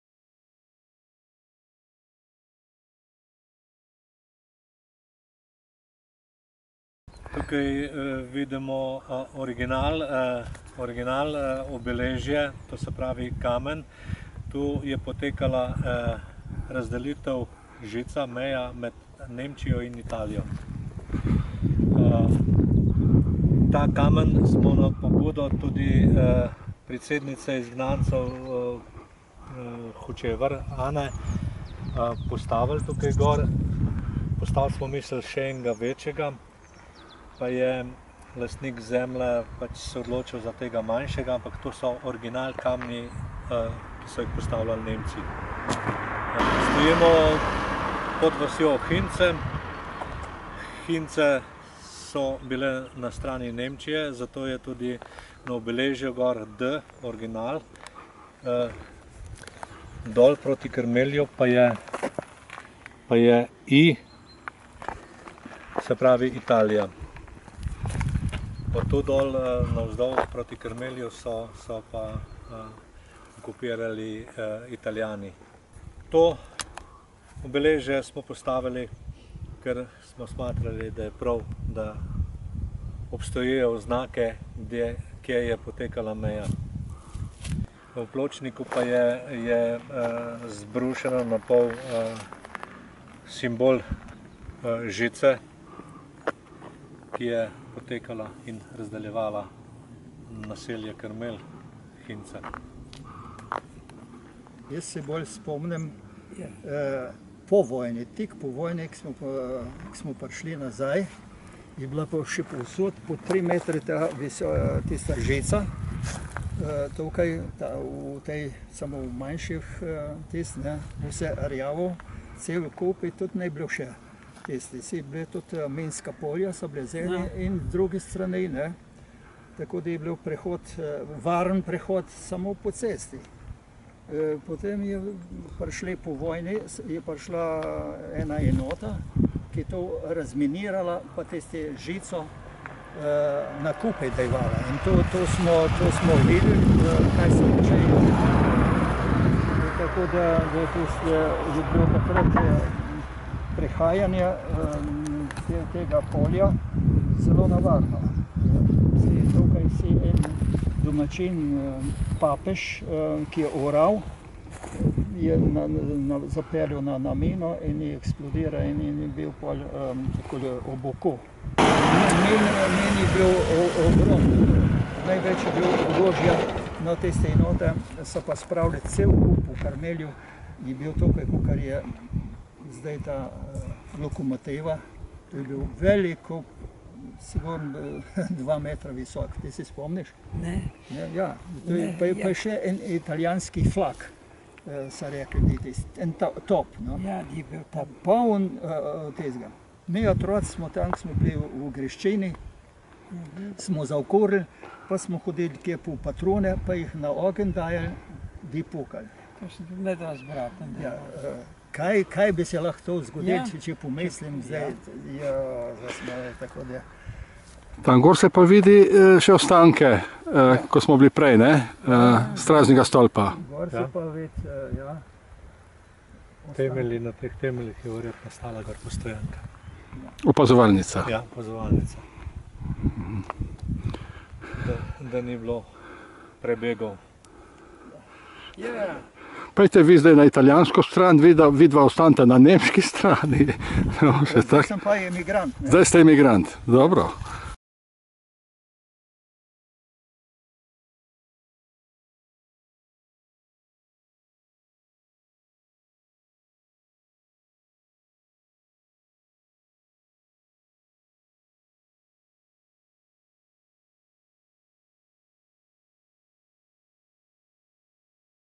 0128 CIRIL KRIŠTOF, RAFKO BLAŽIČ, MARJAN ZAMAN, HINJCE
Marjan Zaman, Ciril Krištof in Rafael Rafko Blažič pripovedujejo o spominskem obeležju, ki obeležuje nekdanjo okupacijsko mejo med Nemčijo in Italijo. Hinjce.